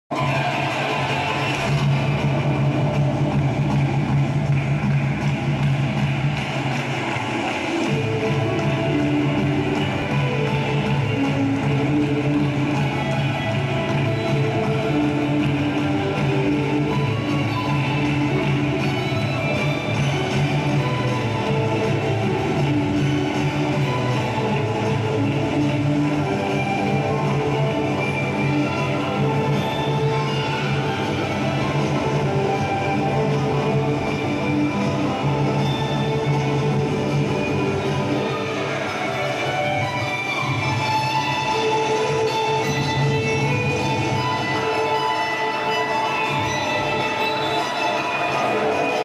Nerea Sueiro Manos libres categoria benjamin escolar
Torneo ekintza. nerea a sido gimnasta del cub ekintza y dana actualmente pertenece al club sakoneta